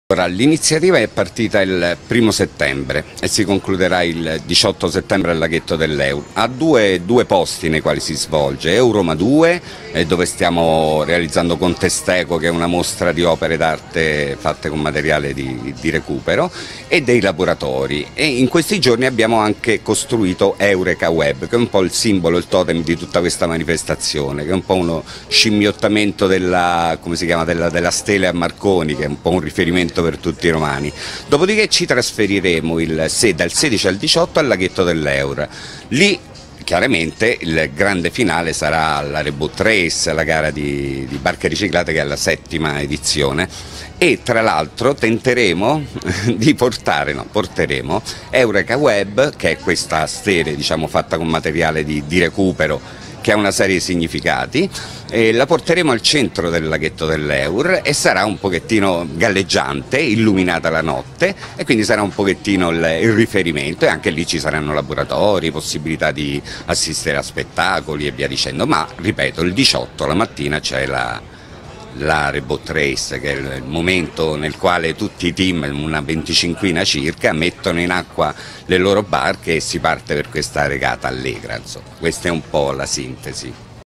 Domenica la Re Boat Roma Race al laghetto dell'Eur
La tradizionale regata con barche costruite con materiali riciclati si svolgerà daò 16 al 18 settembre.

Èliveromatv​ la Capitale Cambia Canale DIRETTE sul CANALE 14 e su